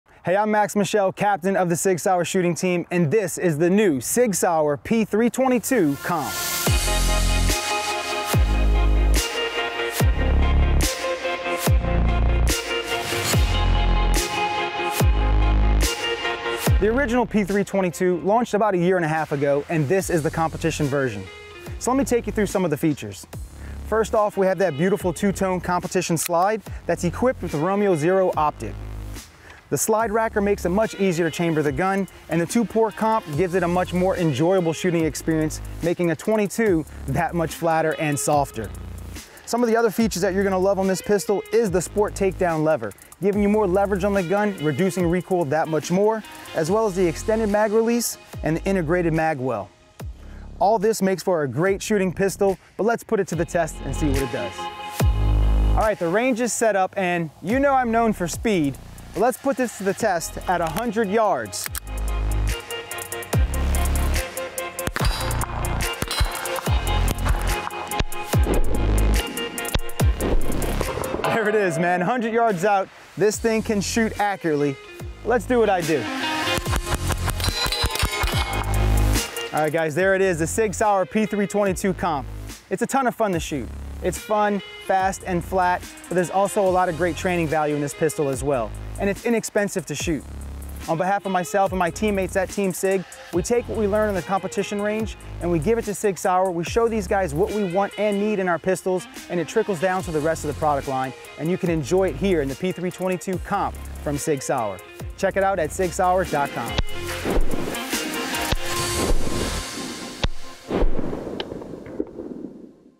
P322-COMP • SIG SAUER • Max Michel
Making hits with a P322-COMP from 100 yards?
No problem for @MaxMichelTV  🏆